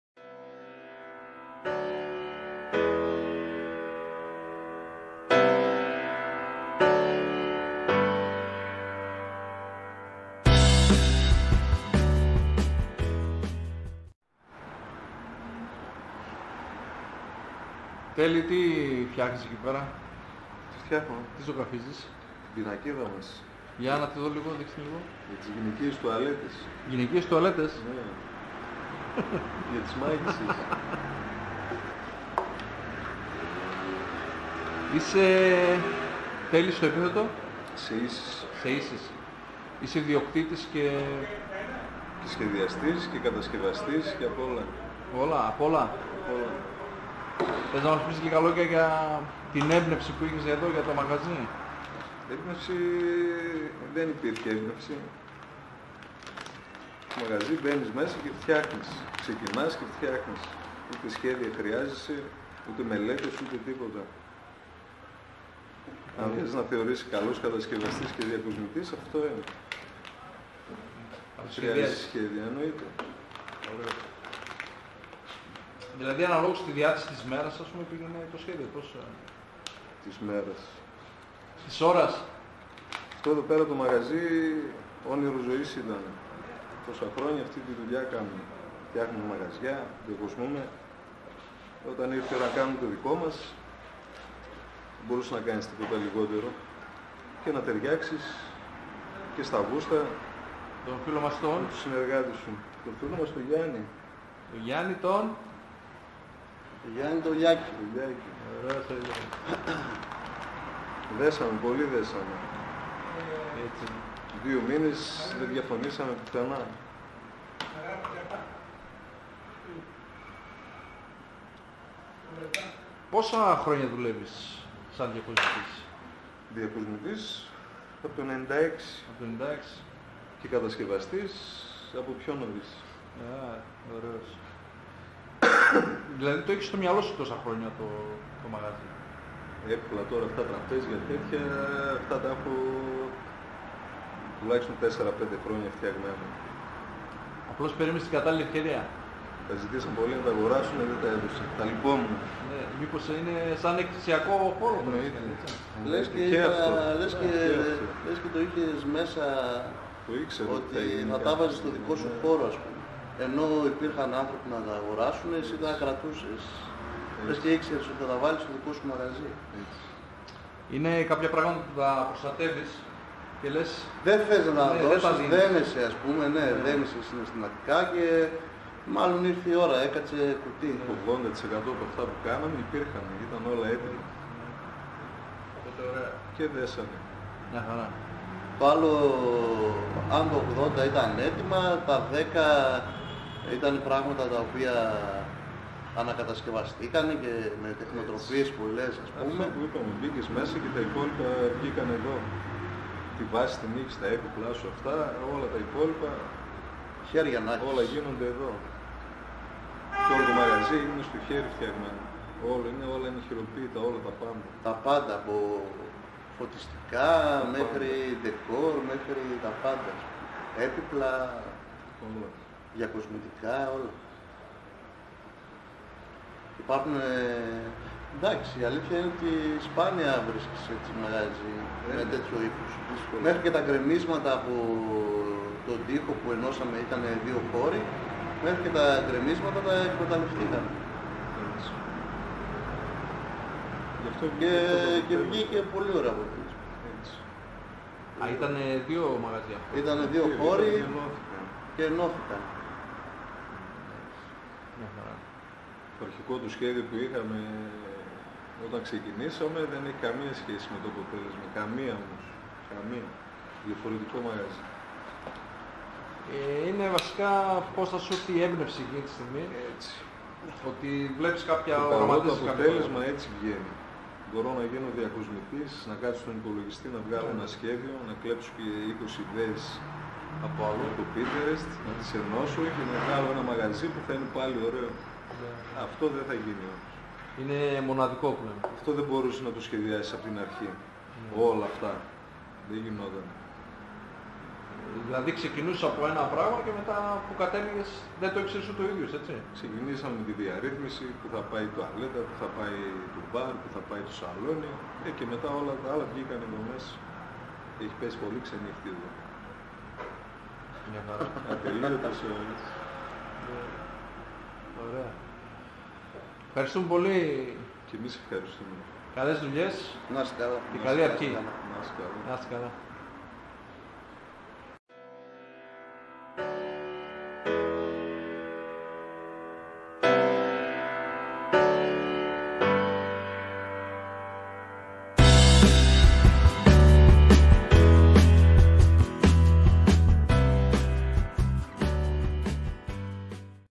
Pato Negro Mediterranean Cuisine
Μια χαλαρή κουβέντα με δύο από τους δημιουργούς του Pato Negro
 τον Γιάννη Λιάκη  και τον Τέλη Σεϊση στα Γιαννιτσά.

Όποιος θέλει να βοηθήσει οικονομικά το κανάλι μας μπορεί να το κάνει στο παρακάτω